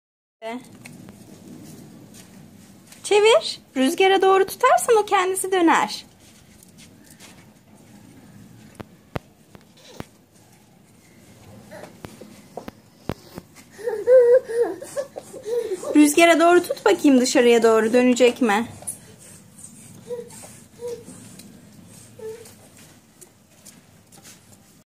Tahtadan rüzgar gülü :)
Babasıyla evdeki tahta parçalarını değerlendirmek adına yaptıkları bu etkinlikte, kızım rüzgar gülünu çok sevdi. Aybike'nin Rüzgarın gücünü hissedebilecegi ve öğrenebilecegi bu etkinlikle doğayı kesfetmiş oldular.